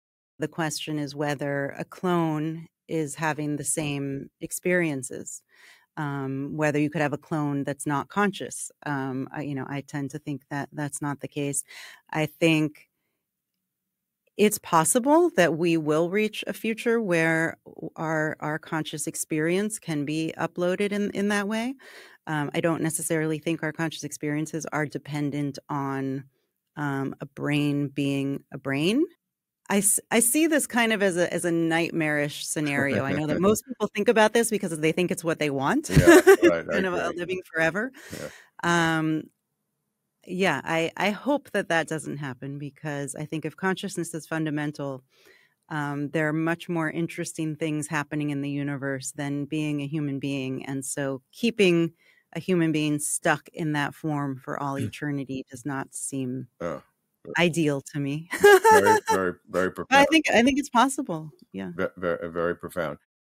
Can clones be conscious?
Annaka Harris discusses the "nightmare" possibility of clones with a consciousness. Watch our new interview with her premiering Tuesday, March 18 at 11am ET